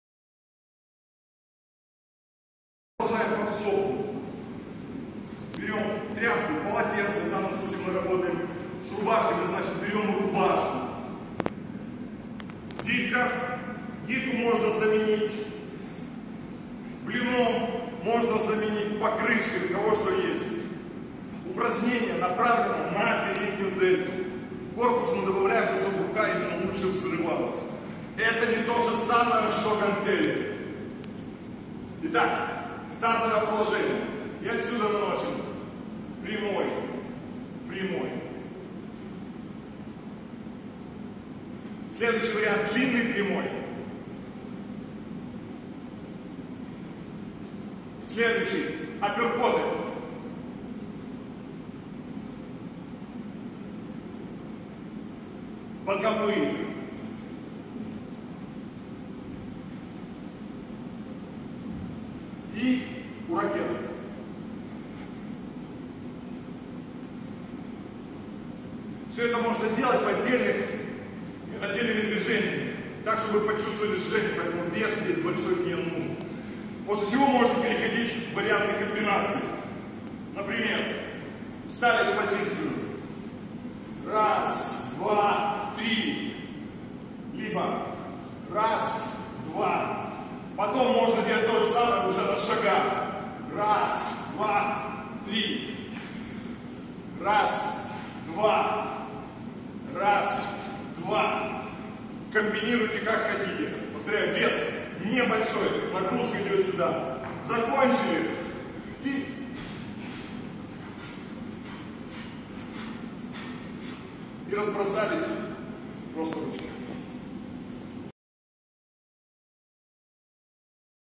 Подсобка с гирей для удара. Руслан Матвеев.
В данном варианте вся нагрузка качественно переходит на переднюю дельту, что и способствует взрывному старту при нанесении удара. Это не тоже самое, что и гантели или утяжелители. Совсем другая нагрузка.